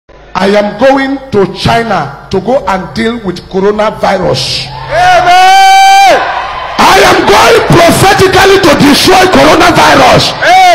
A Prophet Declares in his Church to travel to China to deal with CORONAVIRUS
He actually declared this during prophecy and deliverance moment in his church.